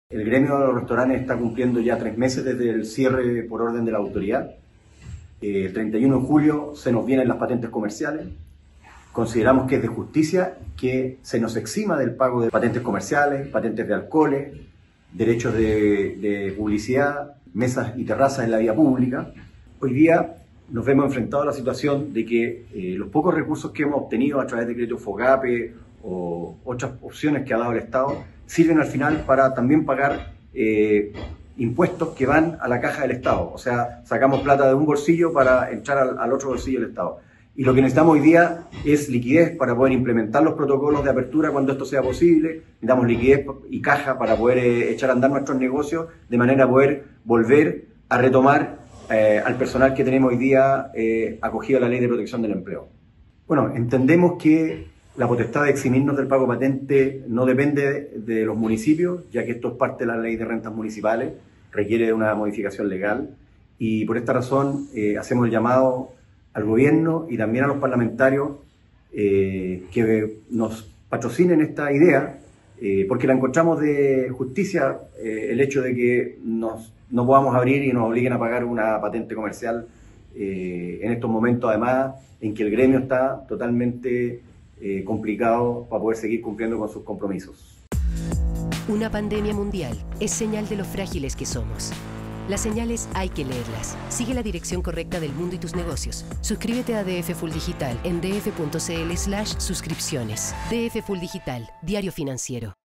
Ahora en DF: Achiga pide eximir al sector gastronómico del pago de patentes comerciales
Desde la Asociación Chilena de Gastronomía (Achiga) aseguran que, pese a que existen algunos locales entregando servicios vía delivery, esto no cubre ni el 15% de los ingresos percibidos en tiempos normales.